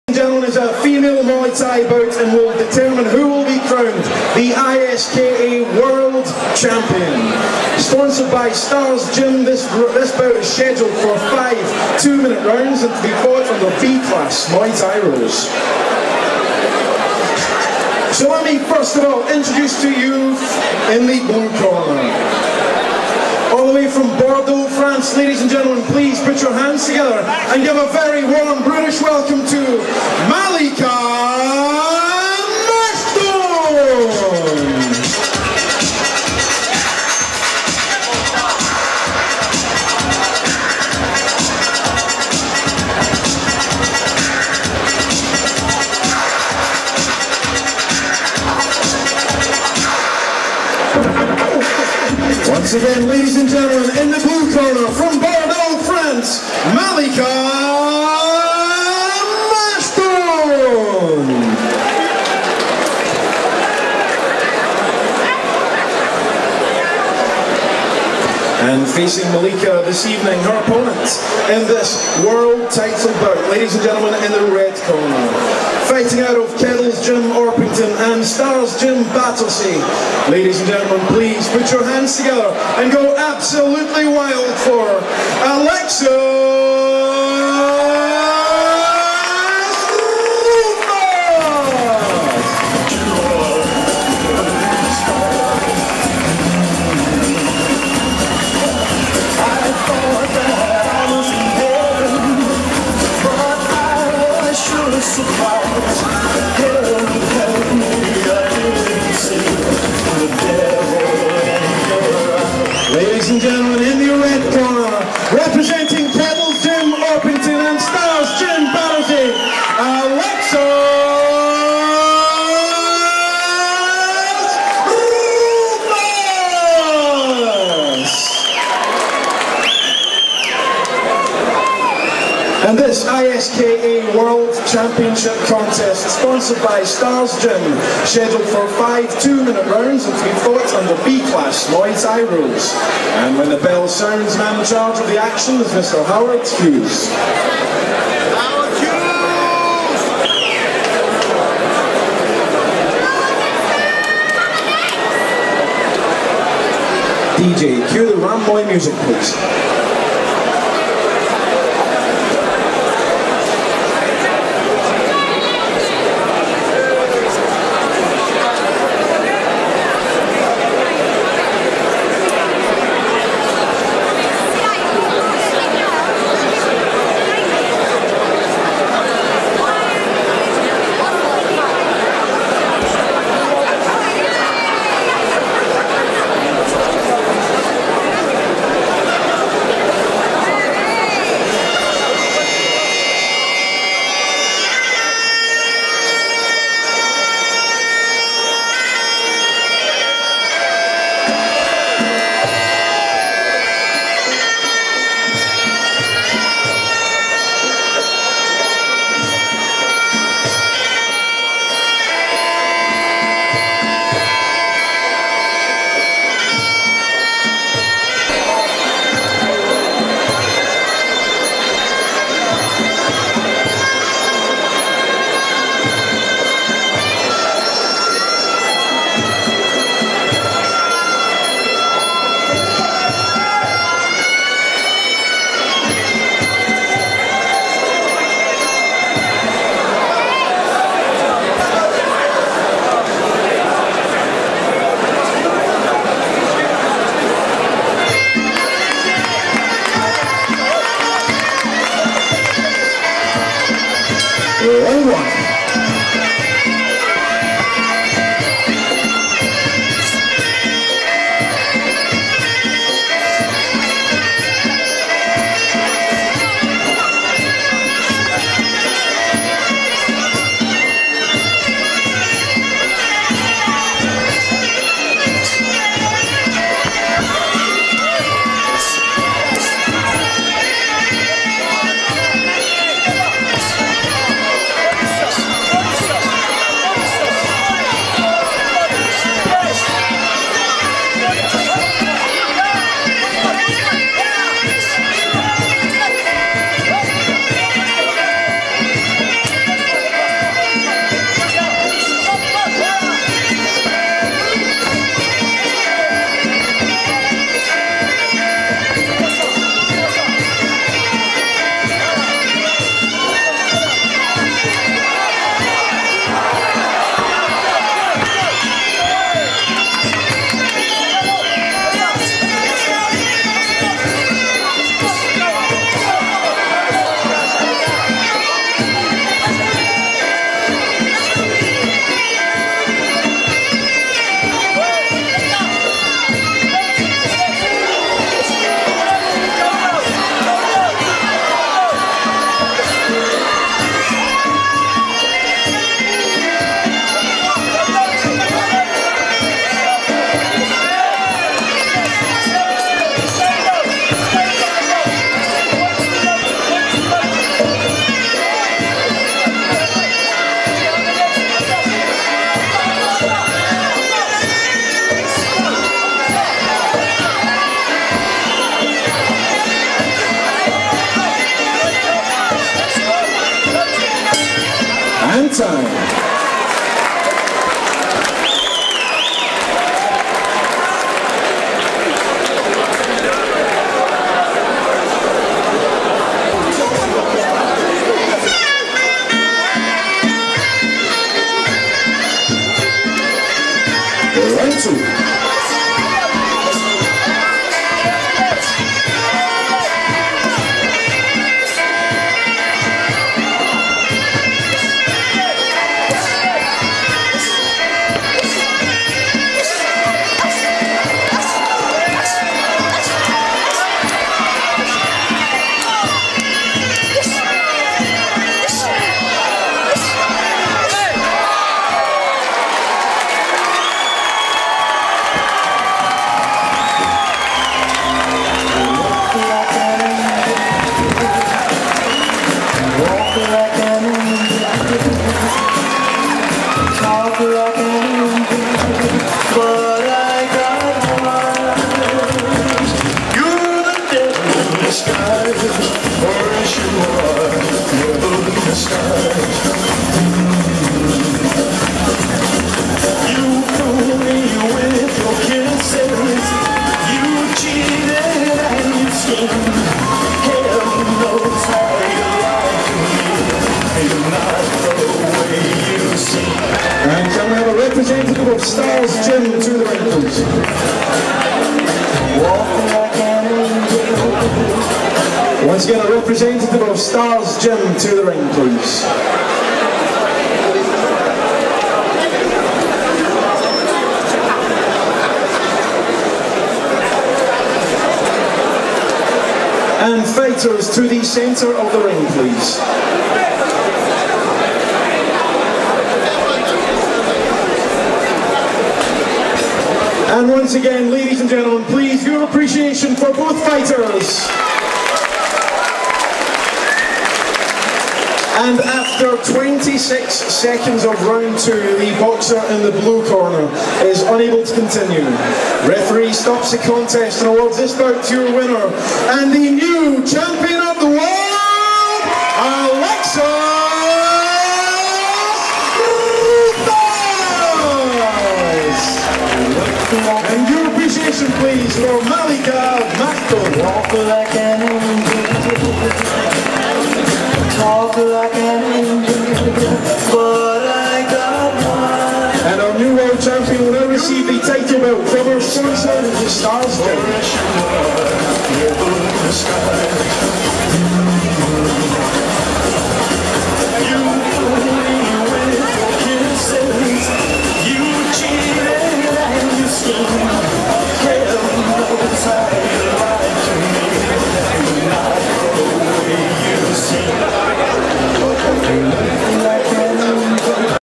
Alexis Rufus vs Malika Machtoune
Keddle's Promotions Presents an Evening of Thai Boxing, MMA and Boxing.
Saturday 23rd Octoboer 2010 @ The City Pavilion, Collier Row Road, Romford, Essex, RM52BH